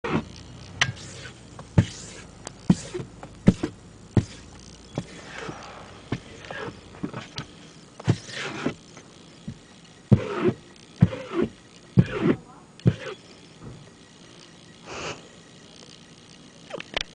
MY DAD SQUEEGIES THE WINDOW
JULY 31, 2007 at the gas station with my dad.  He thinks I'm taking a picture of him, doesn't know it's a video!